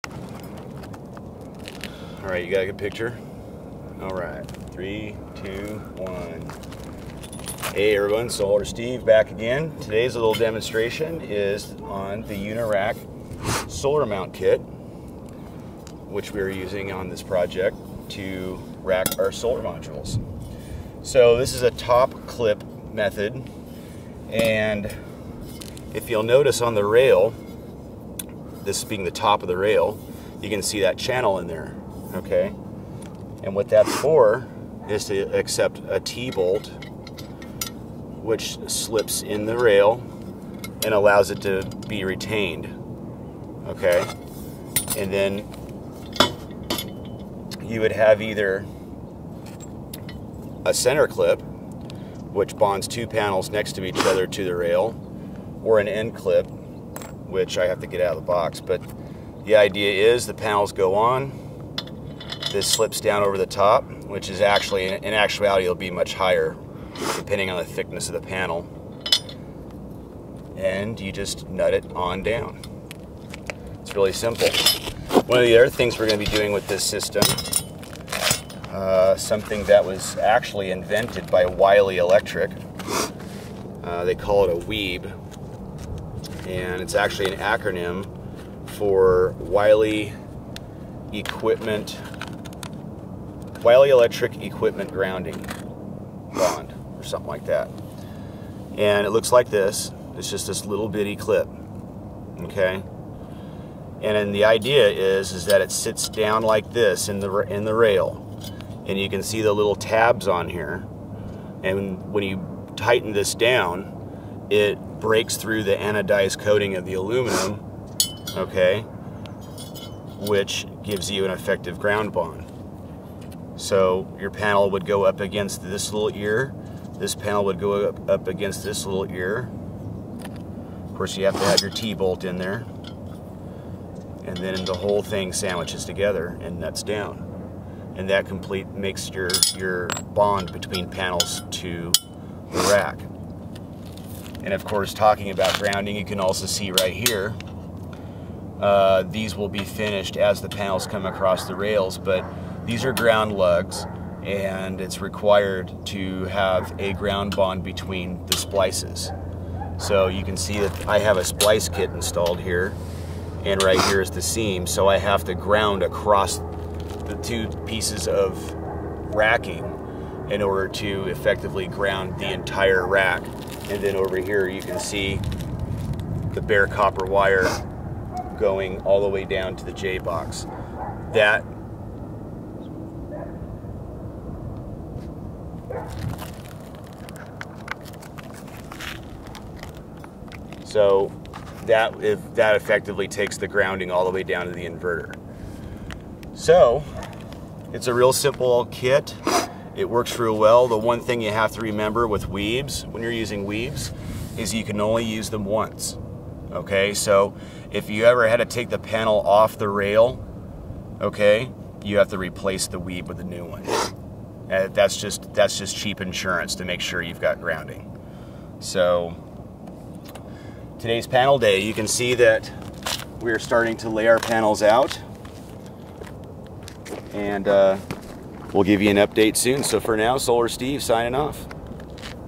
Unirac Solarmount Racking System for Gridtie Project
I demonstrate the Unirac Solarmount System i used in my gridtie project